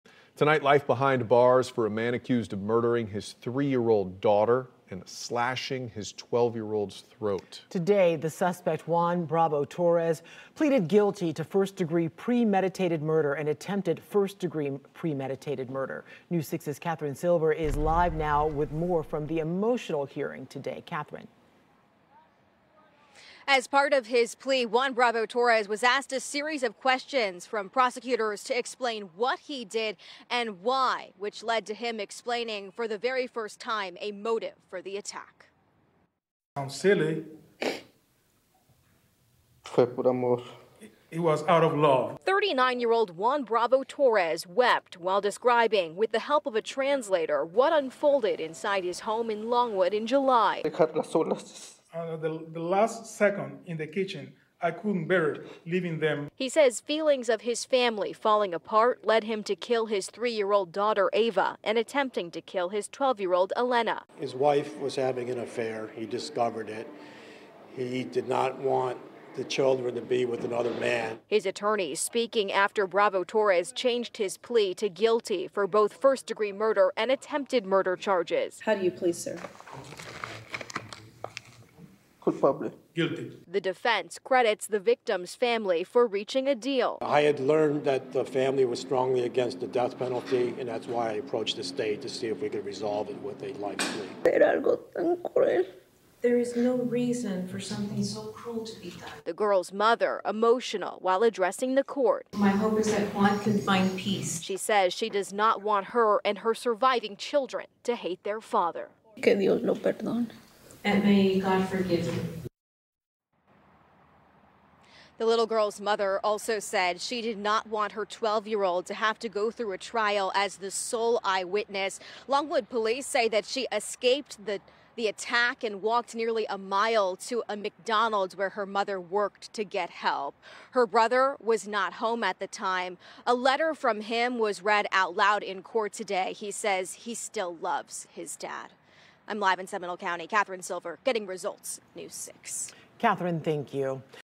Longwood father pleads guilty after killing 3-year-old, slashing daughter's throat
A Longwood father accused of killing his 3-year-old daughter and slashing his 12-year-old’s throat at their home in July 2022 entered a guilty plea Wednesday afternoon.